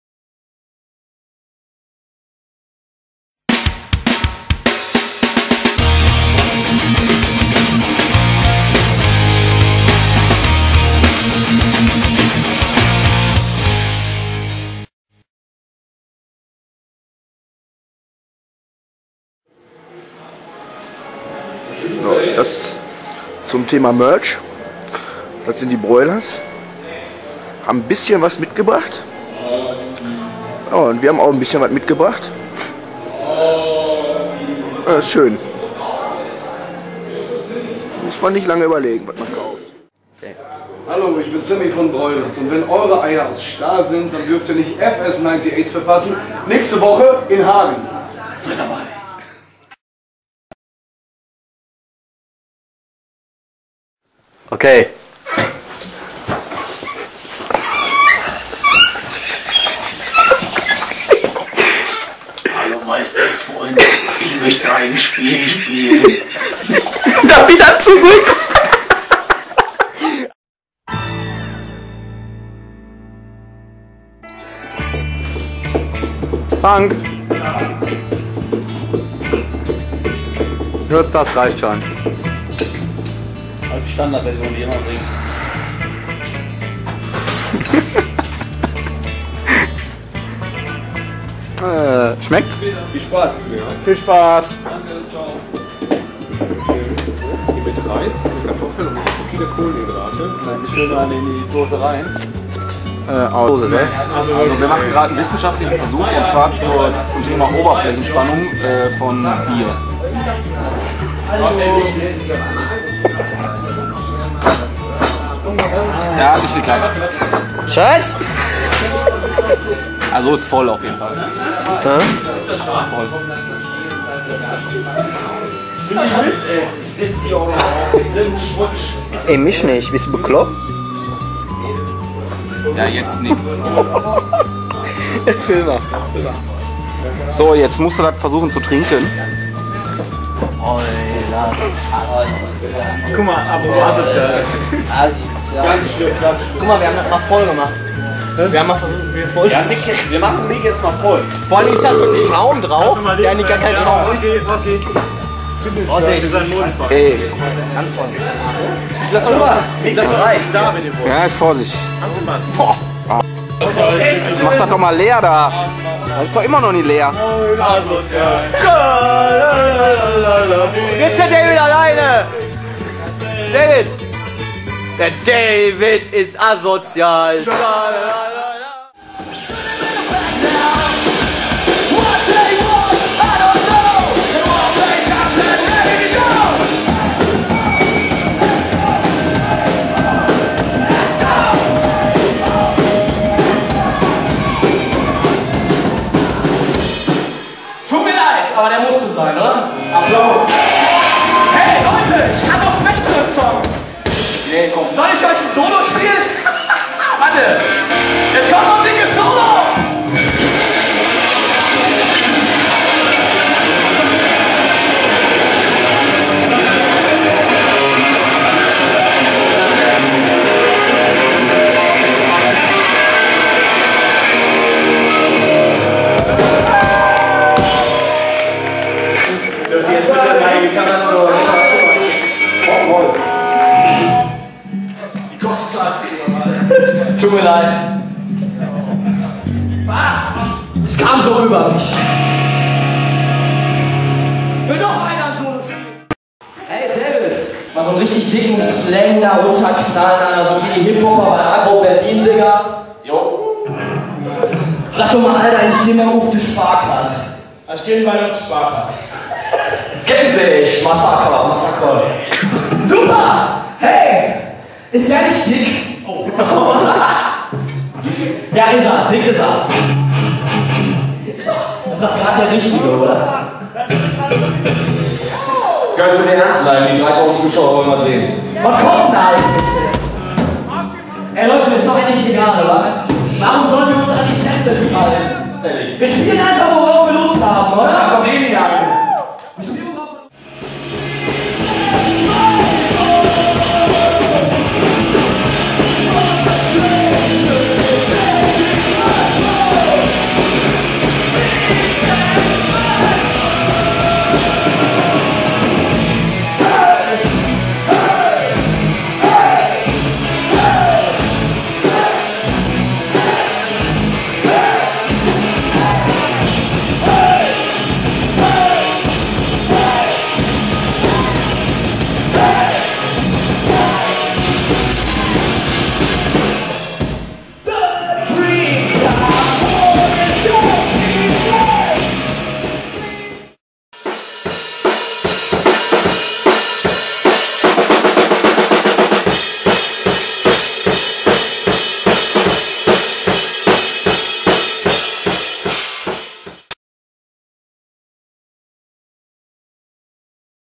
fs-ninety8 TV Vol.20
fs-ninety8 Live im Kultopia Hagen. Backstage Quatsch. Nik spielt mitlerweile auch schon ein Solo. david ist Dick und pennt!!!
All das in Folge 20. Enjoy it